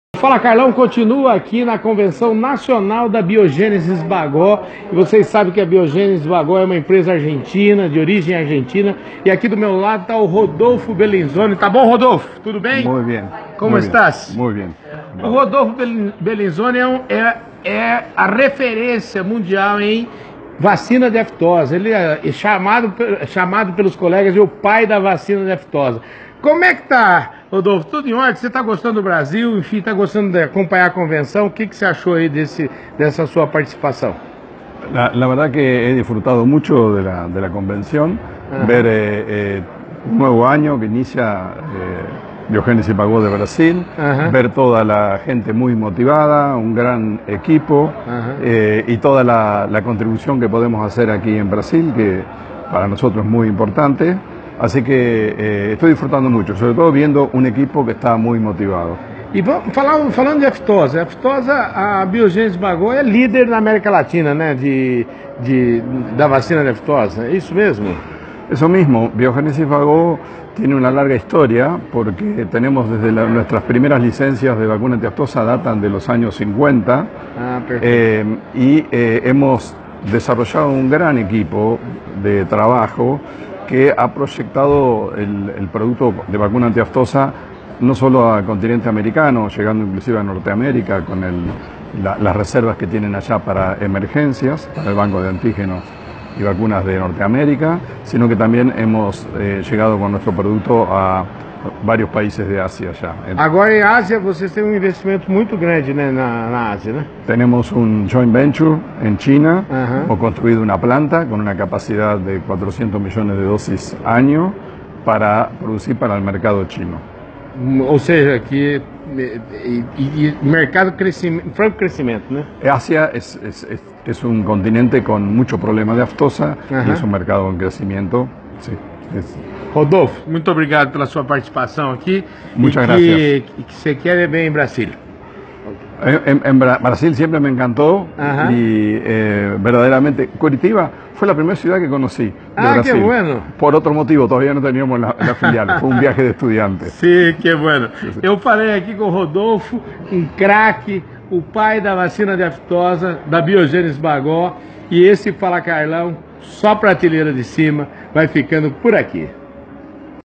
Fala Carlão | Edição 1305 conversa com Rodolfo Belinzoni, Diretor Industrial da Biogénesis Bagó
Fala Carlão, edição 1305 conversa com Rodolfo Belinzoni, Diretor Industrial da Biogénesis Bagó aqui na Convenção Nacional da empresa. Ele é o pai da vacina de aftosa. Acompanhe a prosa.